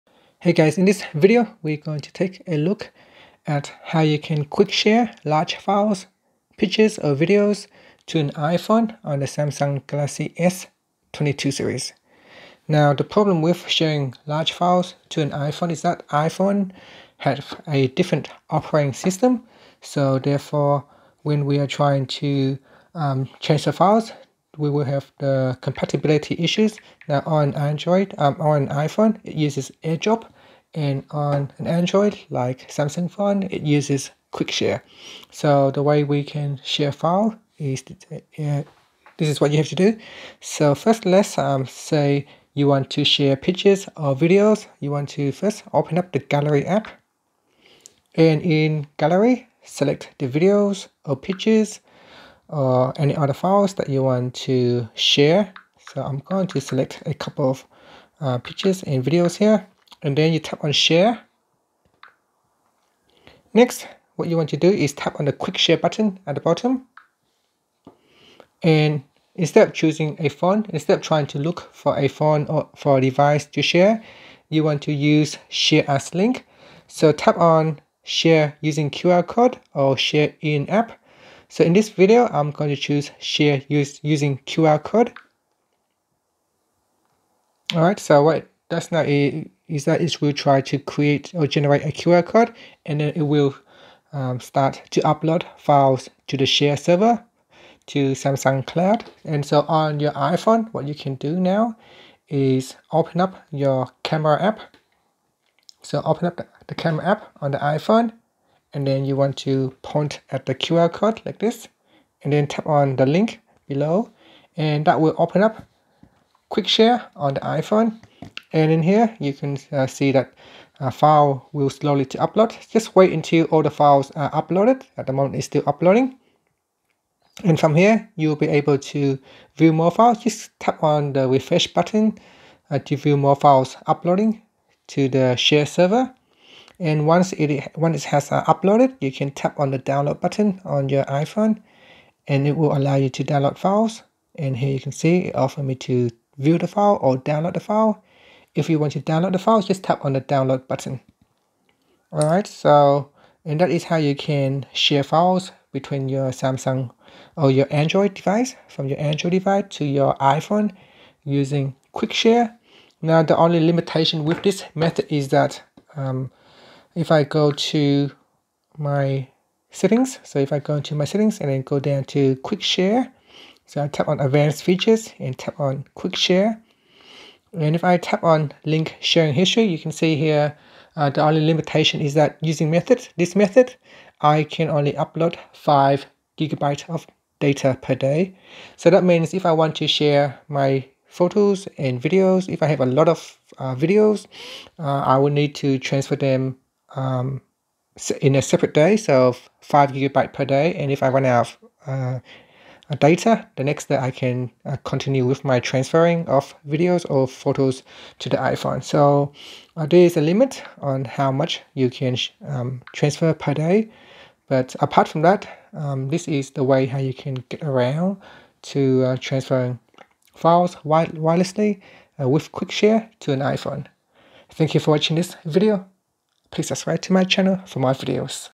How to Quick Share Files/Pictures/Videos from Samsung Android to an iPhone
Learn how you can Quick Share files, pictures, videos to an iPhone on Samsung Android, Samsung Galaxy S22/S22+/Ultra.